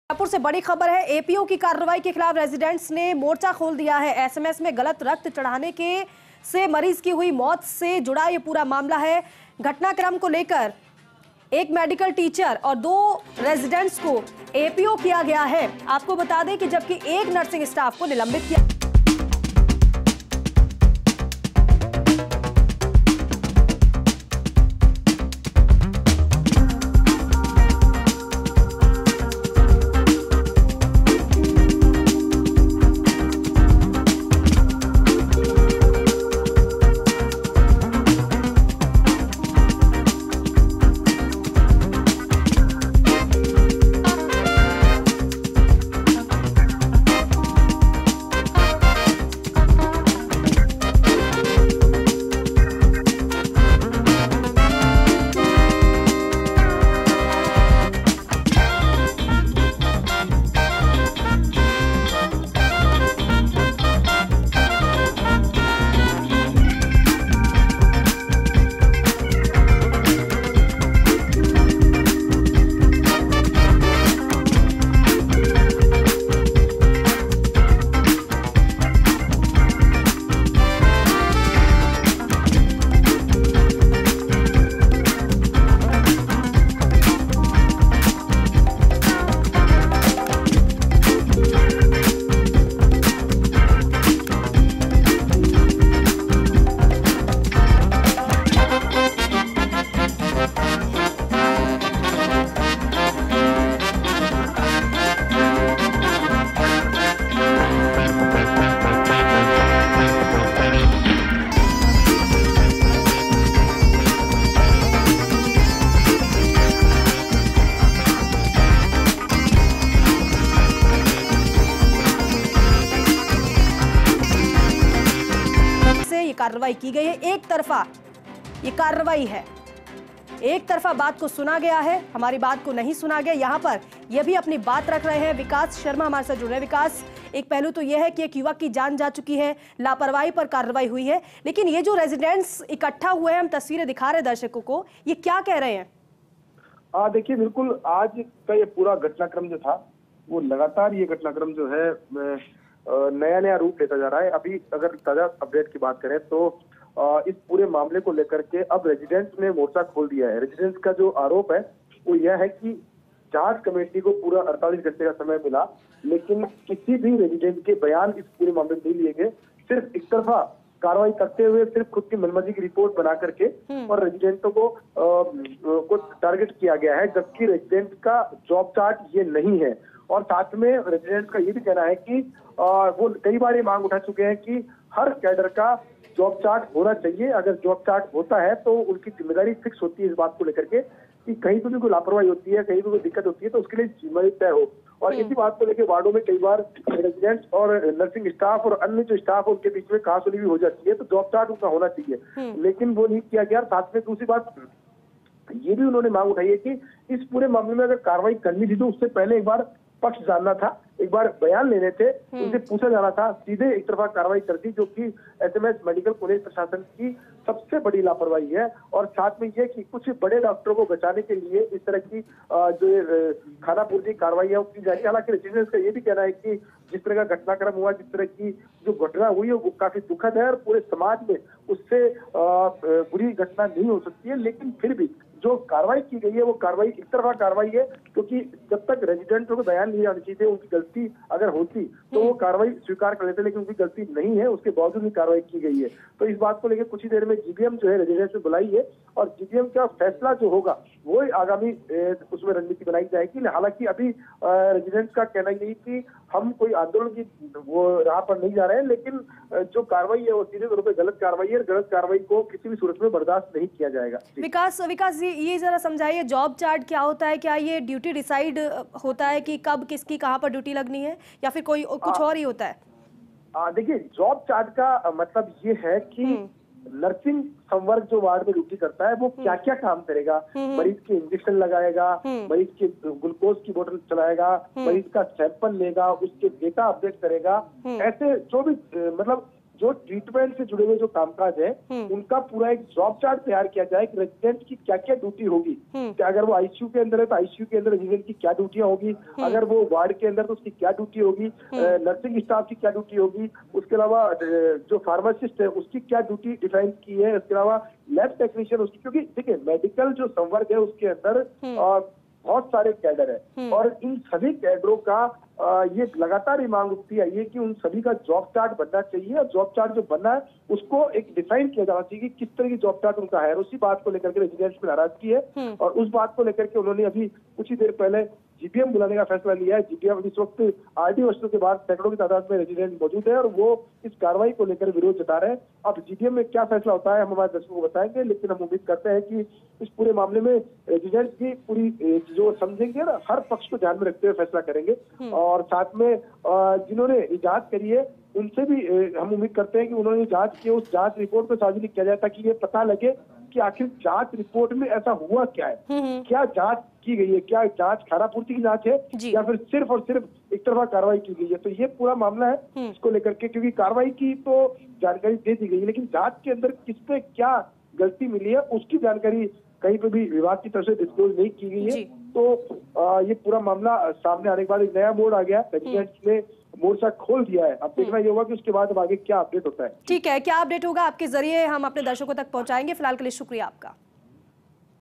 गलत खून चढ़ाने से मौत मामले में APO की कार्रवाई के खिलाफ रेजीडेंट्स ने खोला मोर्चा | Rajasthan News
About this Video:

जयपुर,APO की कार्रवाई के खिलाफ रेजीडेंट्स ने खोला मोर्चा ,SMS में गलत रक्त चढ़ाने से मरीज की हुई मौत से जुड़ा प्रकरण ,घटनाक्रम को लेकर एक मेडिकल टीचर और
दो रेजीडेंट्स को किया गया है APO,जबकि एक नर्सिंग स्टाफ को किया गया है निलंबित
प्रशासन की कार्रवाई को लेकर रेजीडेंट्स में आक्रोश,जार्ड के बैनर तले सभी रेजीडेंट्स RD हॉस्टल के बाहर एकजुट ,प्रशासन की कार्रवाई को बताया एकतरफा ,रेजीडेंट्स का आरोप-
'कई बार उठा चुके हैं जॉब चार्ट डिसाइड करने की मांग','लेकिन अभी तक जॉब चार्ट तय नहीं होने से हो रही इस तरह की मनमानी कार्रवाई',पूरे घटनाक्रम पर आगामी रणनीति तय करने के लिए अभी 10:30 बजे बुलाई गई GBM , | 23 February 2024